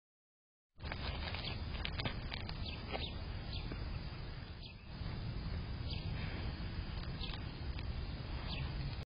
Deer close-up caught eating bird food on Bird Feeder Watch Cam#shorts
Deer close-up caught eating bird food on Bird Feeder Watch Cam@MyWildlife Habitat